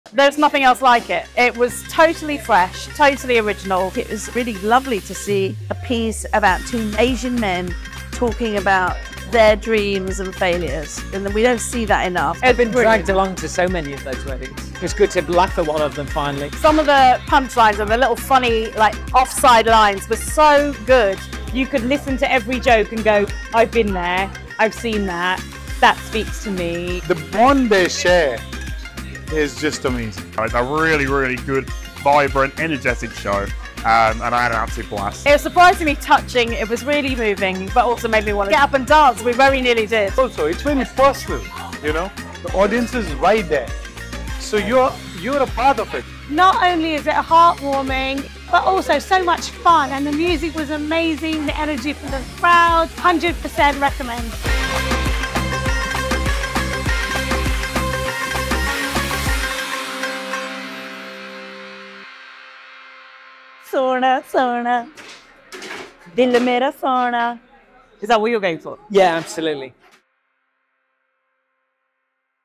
DJs landscape
The critically acclaimed British South Asian theatre company Rifco Theatre come to the Octagon next month. Following a successful run at the Edinburgh Fringe, Rifco Theatre's resident DJ duo, ‘Pali & Jay,’ are hitting the road and they are inviting you to experience the behind-the-scenes of a desi disco party with Pali & Jay’s Ultimate Asian Wedding DJ Roadshow.